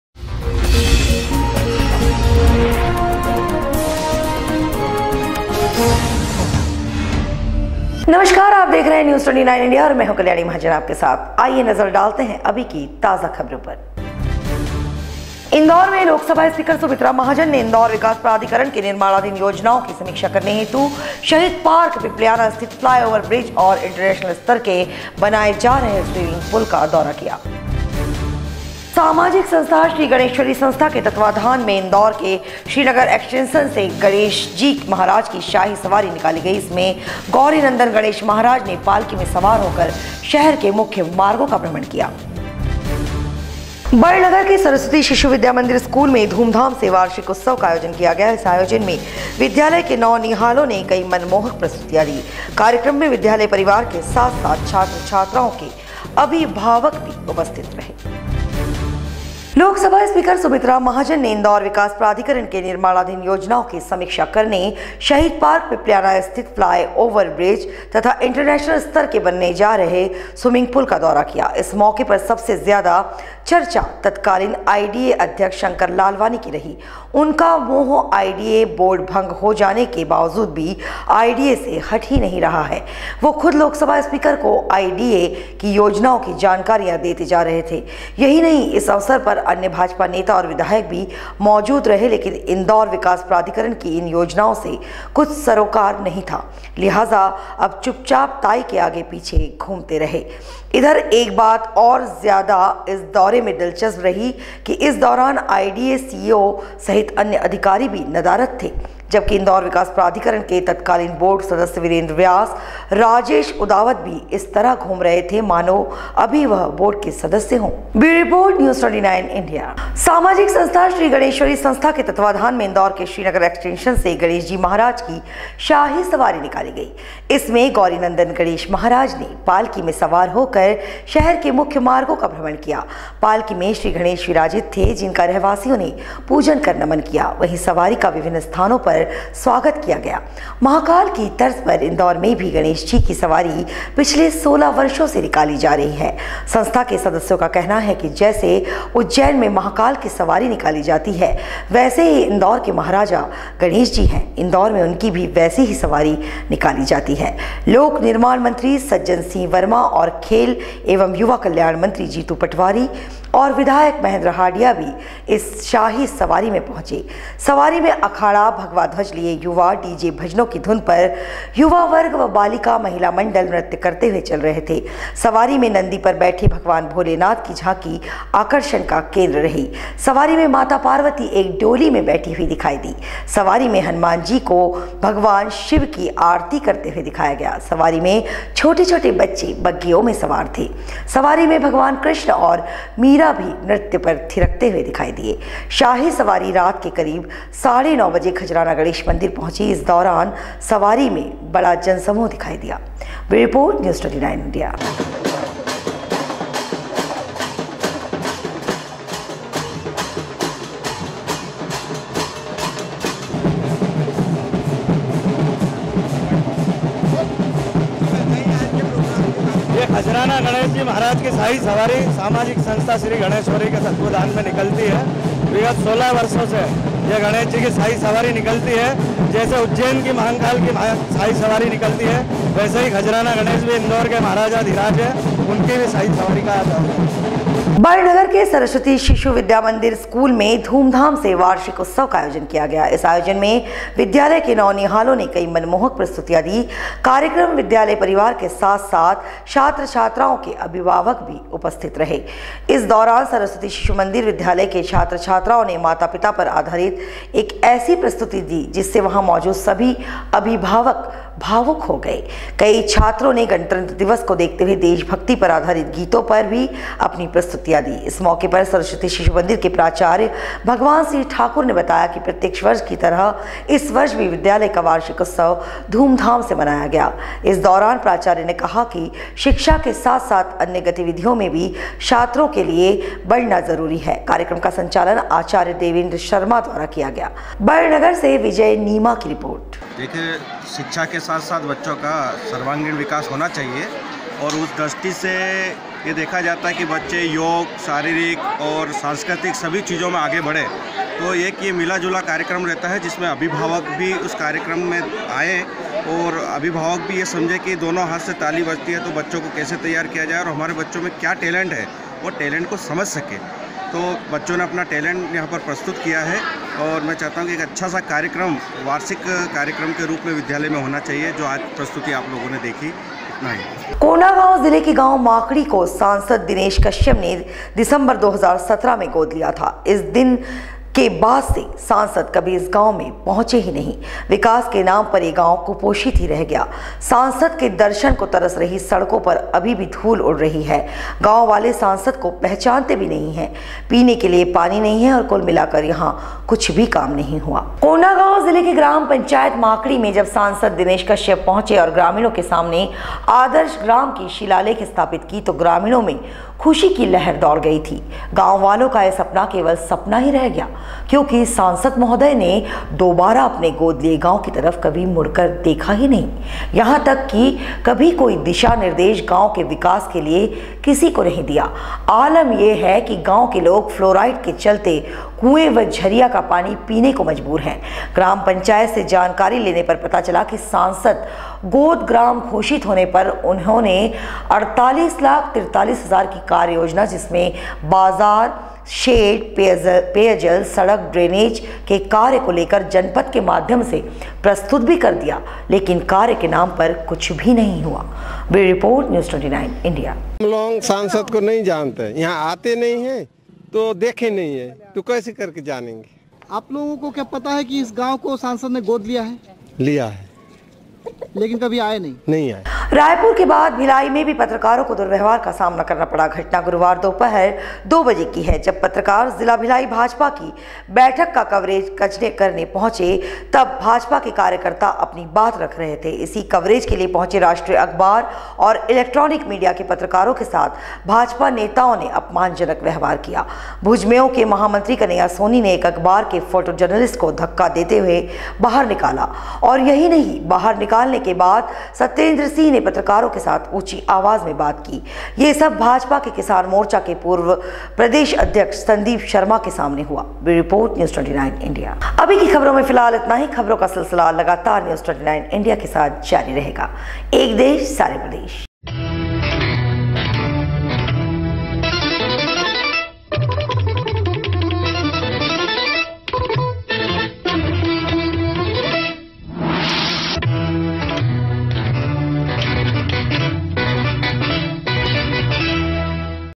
News29India Bulletin 24 Jan lot 6 पत्रकारों को करना पड़ा दुव्र्यवहार का सामना
1 इंदौर में लोकसभा स्पीकर सुमित्रा महाजन ने इंदौर विकास प्राधिकरण के निर्माणाधीन योजनाओं की समीक्षा करने हेतु शहीद पार्क, पिपलियाहाना स्थित फ्लाय ओवर ब्रिज और इंटरनेशनल स्तर के बनाये जा रहे स्विमिंग पुल का दौरा किया

2 समाजिक संस्था श्री गणेश्वरी संस्था के तत्वावधान में इंदौर के श्री नगर एक्सटेंशन से गणेश जी महाराज की शाही सवारी निकाली गई। इसमें गौरीनंदन गणेश महाराज ने पालकी में सवार होकर शहर के मुख्य मार्गो का भ्रमण किया।

3 बड़नगर के सरस्वती शिशु विद्या मंदिर स्कूल में धूमधाम से वार्षिक उत्सव का आयोजन किया गया। इस आयोजन में विद्यालय के नौनिहालों ने कई मनमोहक प्रस्तुतियां दी। कार्यक्रम में विद्यालय परिवार के साथ-साथ छात्र-छात्राओं के अभिभावक उपस्थित रहे।